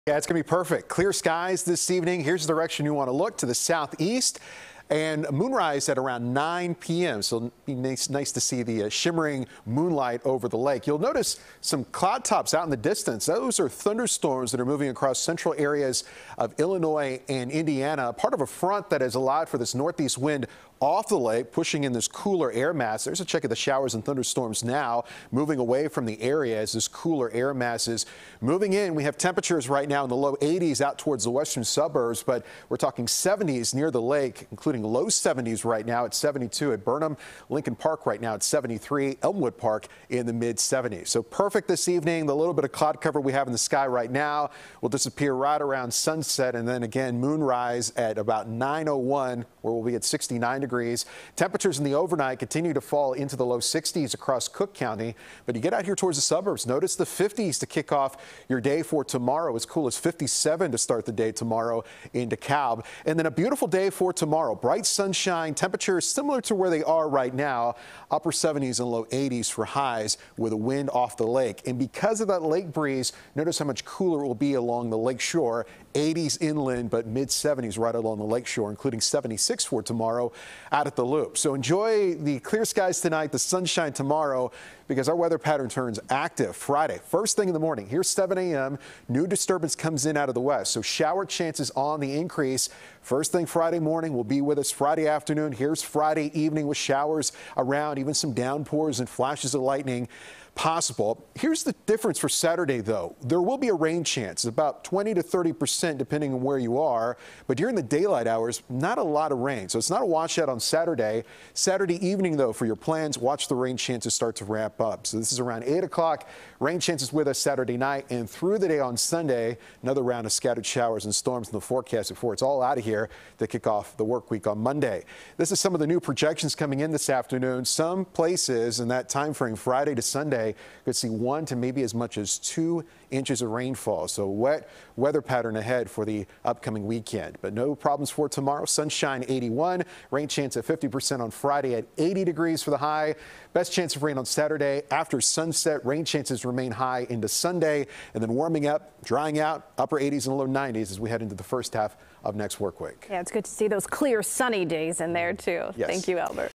Chicago First Alert Weather: All day sunshine
CBS 2 Chief Meteorologist Albert Ramon has a look at the extended forecast.